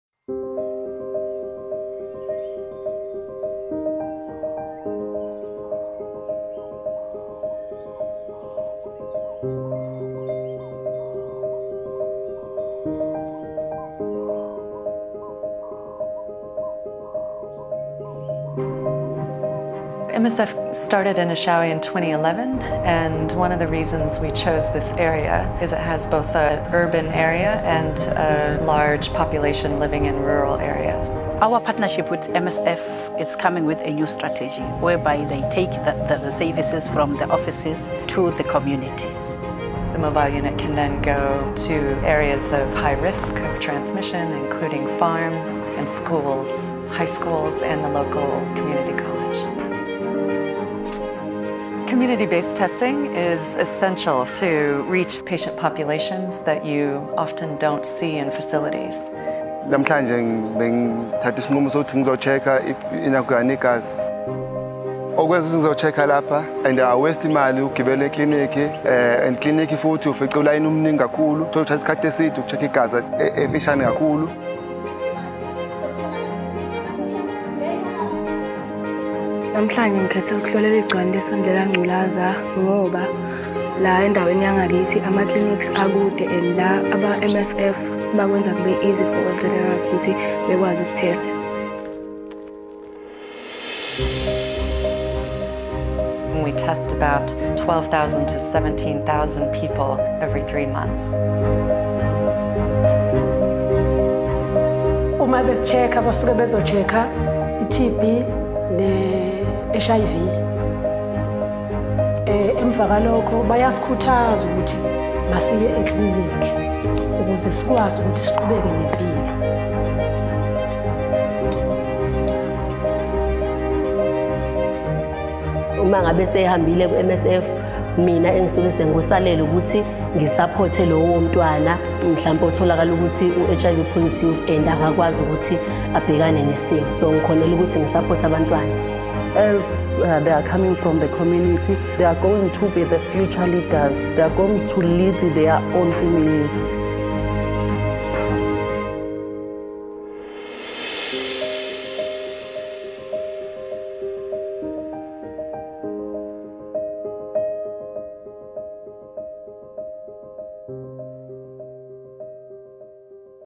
Bending The Curves: The Door-To-Door Fight Against HIV
MSF doctors, counsellors and clients explain why this kind of pro-active outreach testing is critical if South Africa or any similar developing country is to have any hope of reaching the globally agreed 90-90-90 targets. We see MSF nurses and counsellors testing high school learners at their schools, college students at their colleges, farmworkers on the farms where they work, and people in the town who would not go to a health facility for a checkup unless they were very sick.